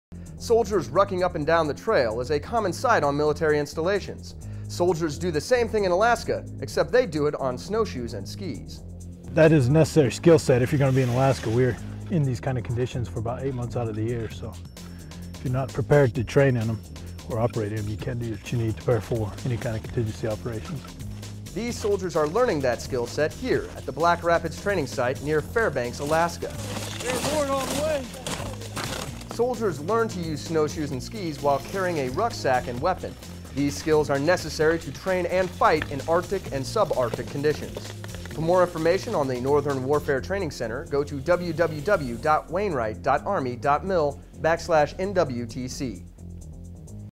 Rucking in the snow
Soldiers learn to use snow shoes and skis while carrying a rucksack and weapon at the Black Rapids Training Site, Northern Warfare Training Center, Alaska. SRTV's Staff Sgt. Justin Puetz reports.